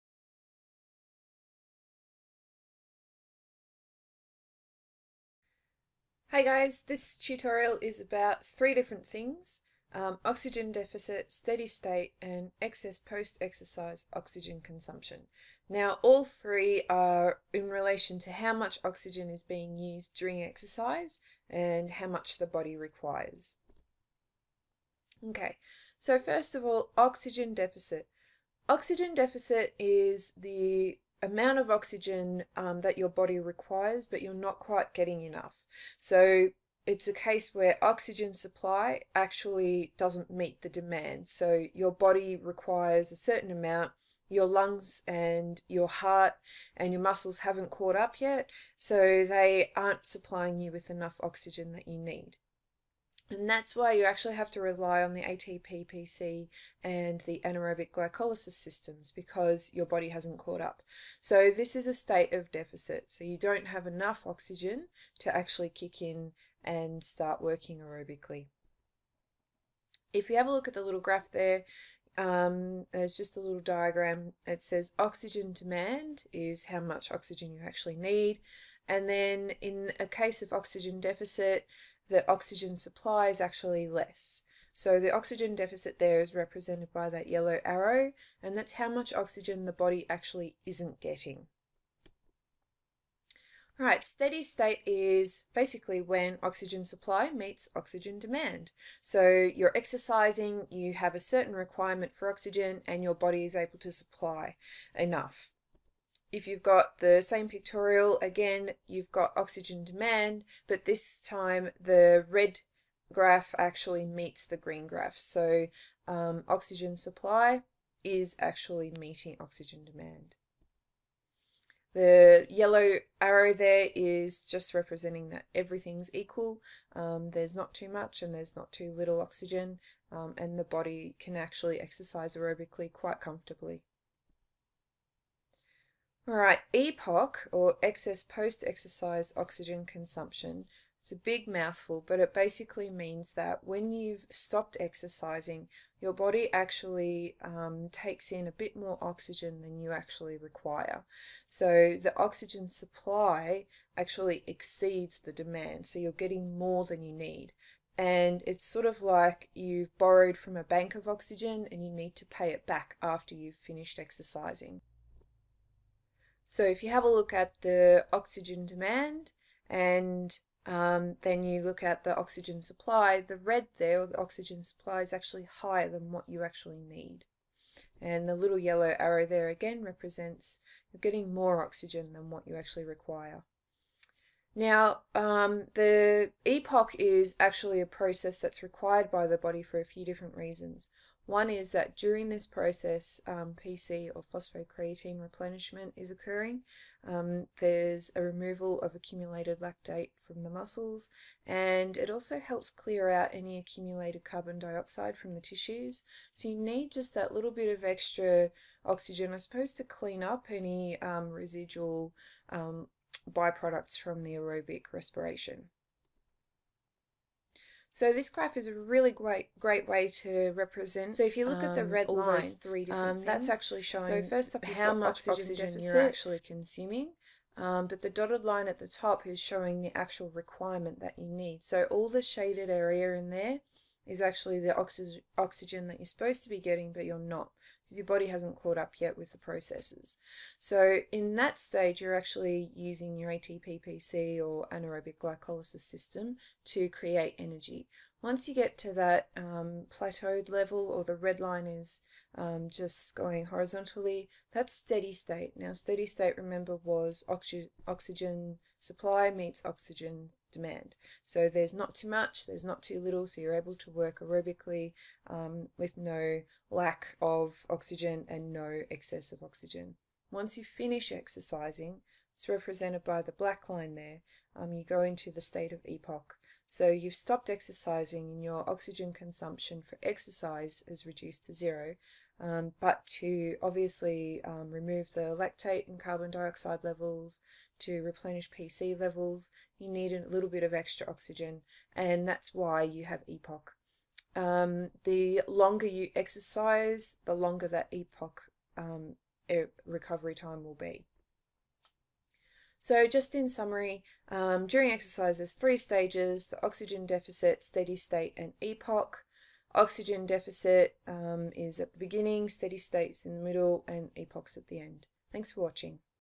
Yr 12 Oxygen use during exercise
Includes oxygen deficit, steady state and EPOC